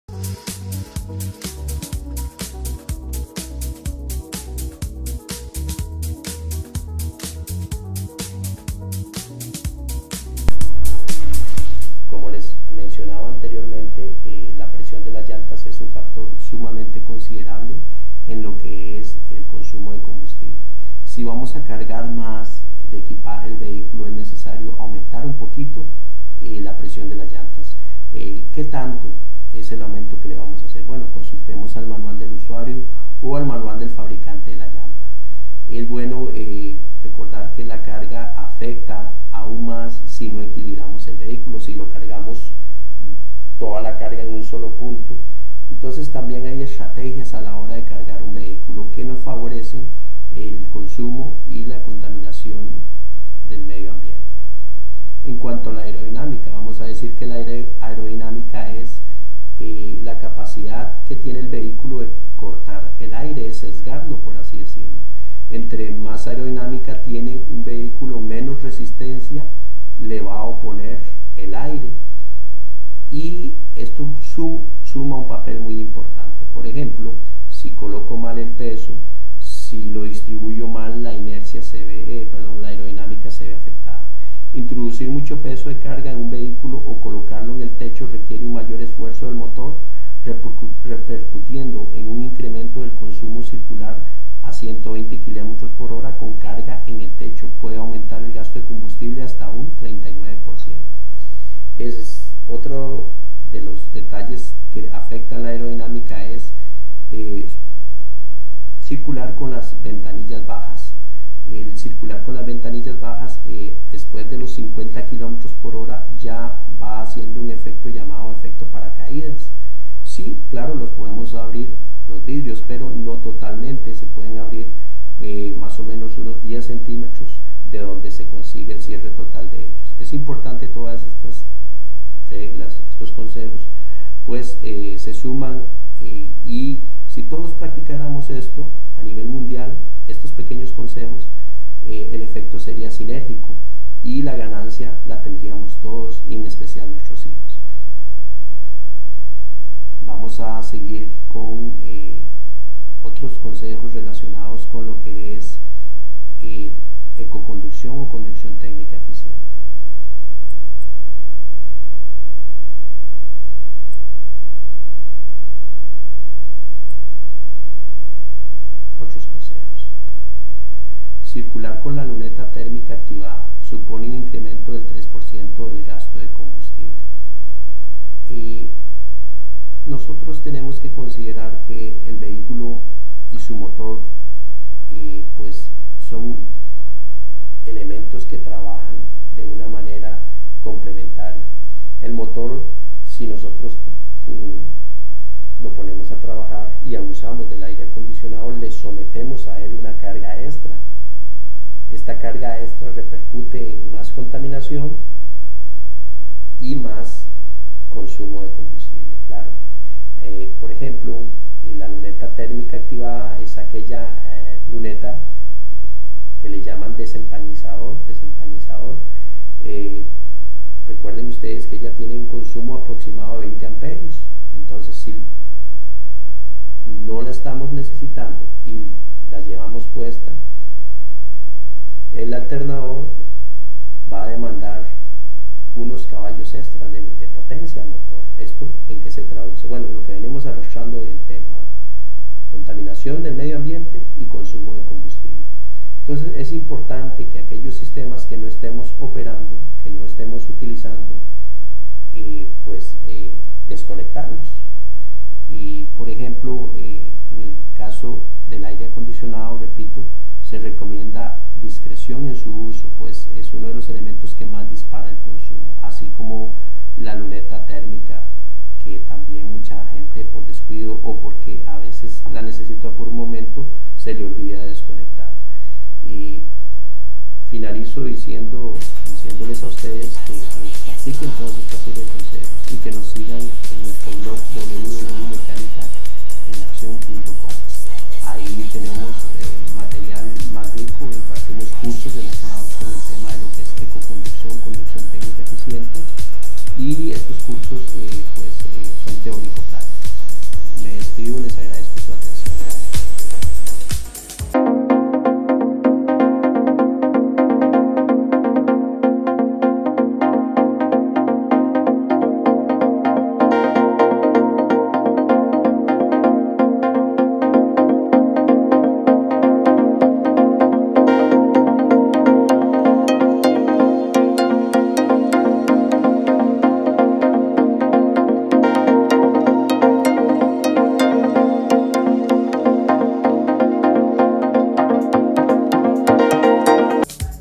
CONSEJO AHORRO COMBUSTIBLE, segunda parte
Antes de iniciar un recorrido conviene vigilar el control de la carga y la aerodinámica del vehículo. Introducir mucho peso de carga en un vehículo o colocarlo en el techo requiere de un mayor esfuerzo del motor repercutiendo en un incremento del consumo.

✅ Te invito a mira la información que te adjunto AquÍ abajo👇

👉 Regístrese en nuestro canal de YouTube. No olvide tocar la "campanita"